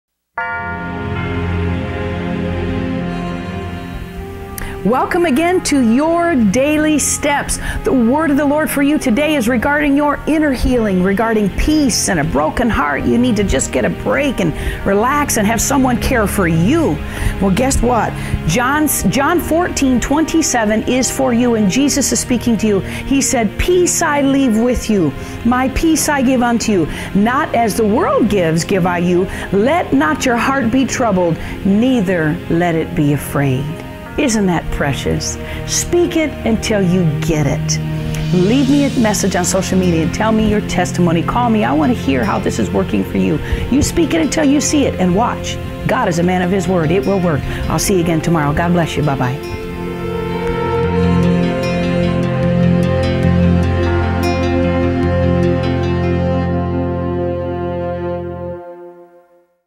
Daily Steps with Dr Gabriel Hope on HEALING! John 14:27
John 14 :27 is another  awesome Bible promise that is available for US to SPEAK and watch manifest! Its about outer AND INNER HEALING... the Lord wants you WHOLE!